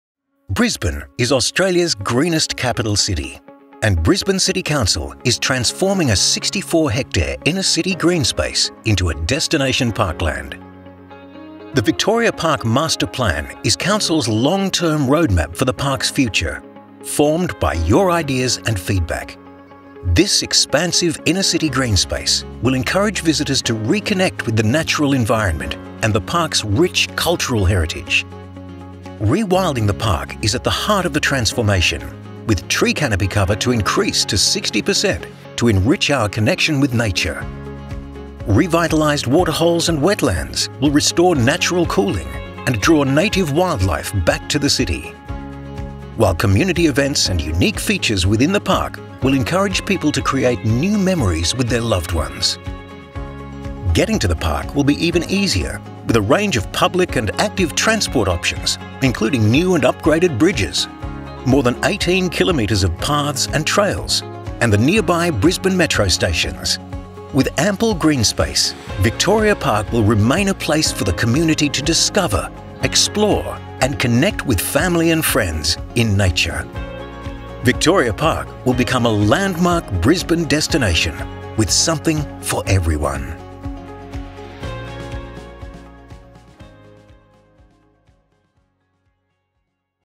Victoria Park / Barrambin Master Plan flythrough
Explore the Victoria Park / Barrambin Master Plan. Victoria Park / Barrambin will become a natural retreat and urban park for adventure, discovery and reconnection.